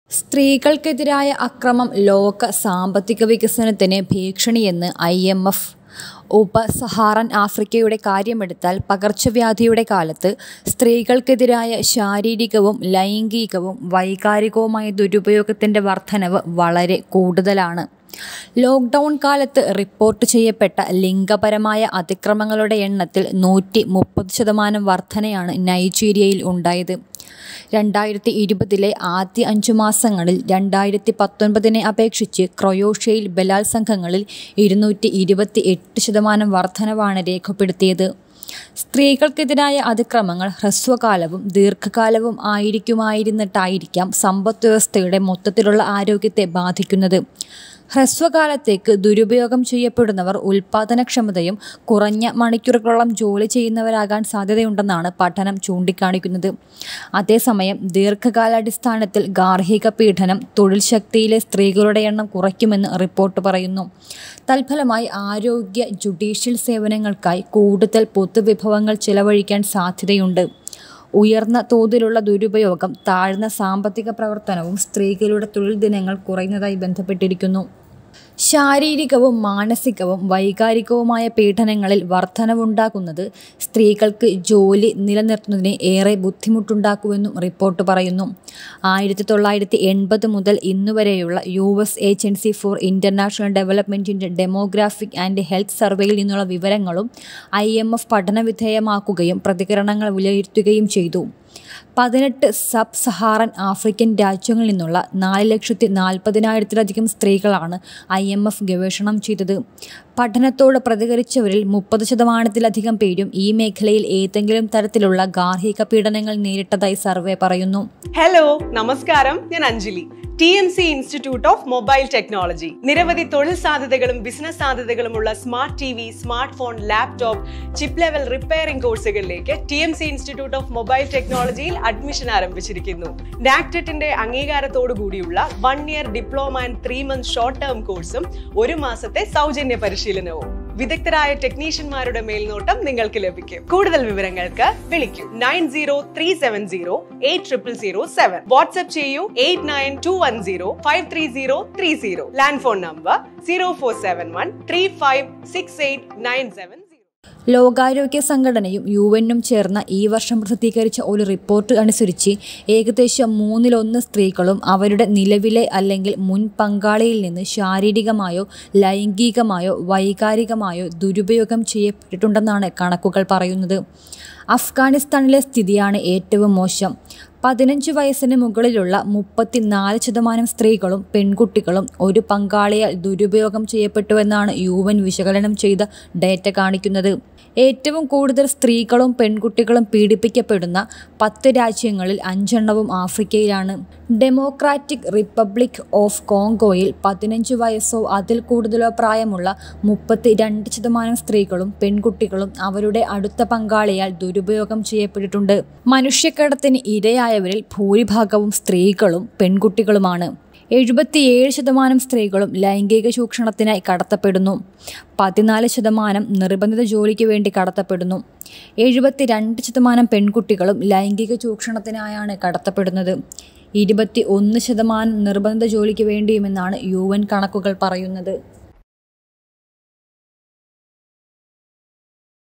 സ്ത്രീകൾക്ക് എതിരായ അക്രമം ലോകസാമ്പത്തിക വികസനത്തിന്‌ ഭീക്ഷണി | Anweshanam
Violence against women is a threat to world economic development.